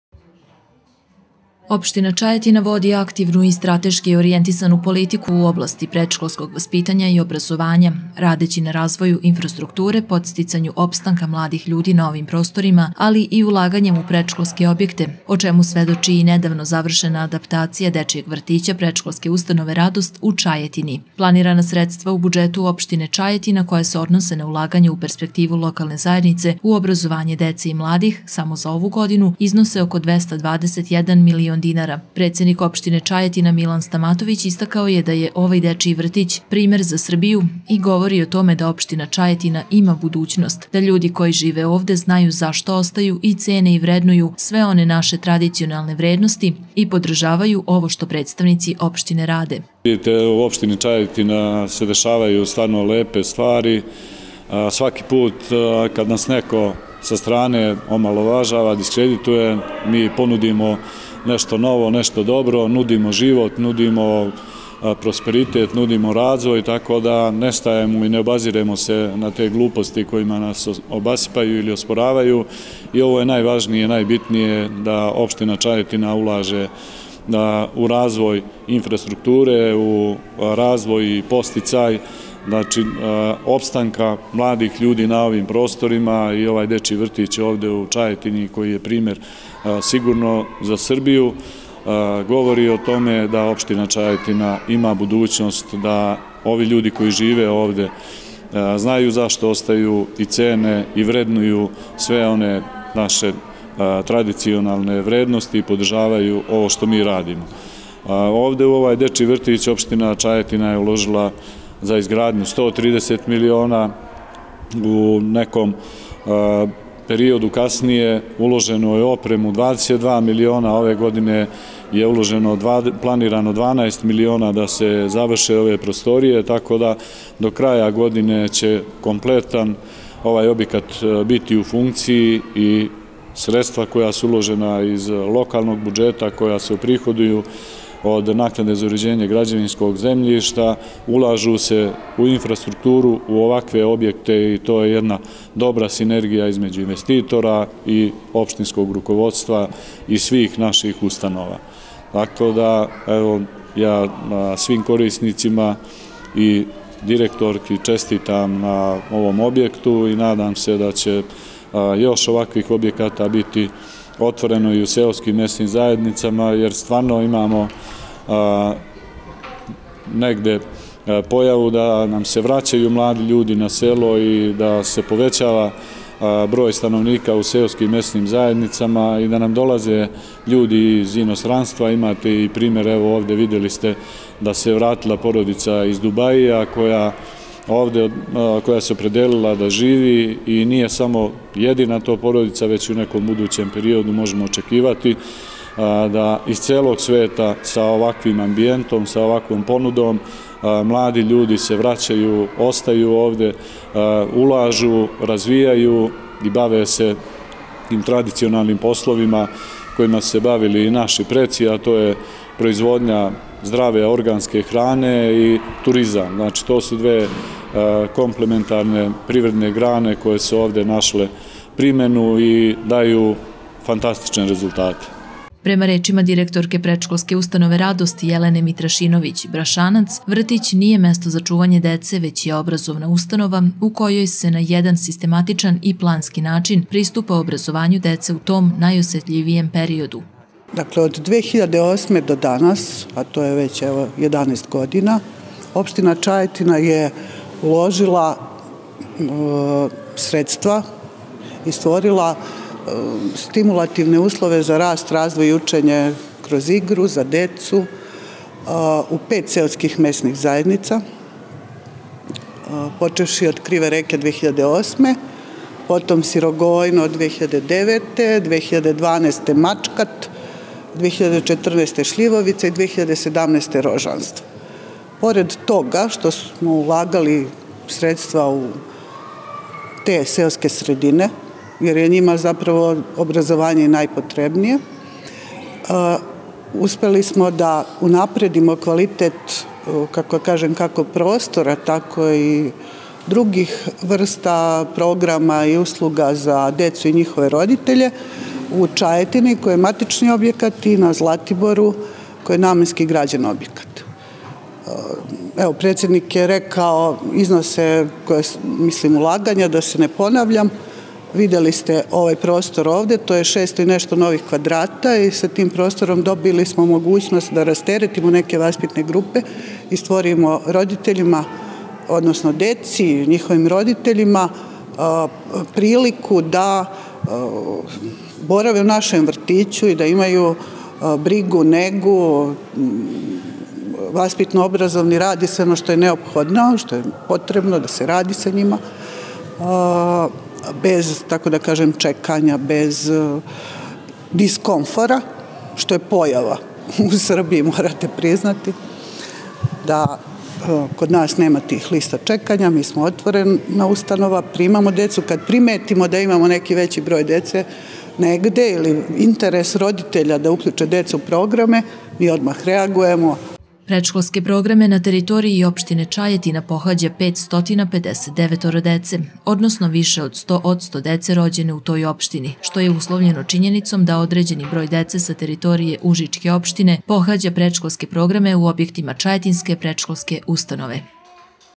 Predsednik opštine Čajetina Milan Stamatović obišao je Predškolsku ustanovu ''Radost'' u Čajetini
Opština Čajetina vodi aktivnu i strateški orijentisanu politiku u oblasti predškolskog vaspitanja i obrazovanja, radeći na razvoju infrastrukture, podsticaju opstanka mladih ljudi na ovim prostorima, ali i ulaganjem u predškolske objekte, o čemu svedoči i nedavno završena adaptacija dečijeg vrtića Predškolske ustanove ''Radost'' u Čajetini.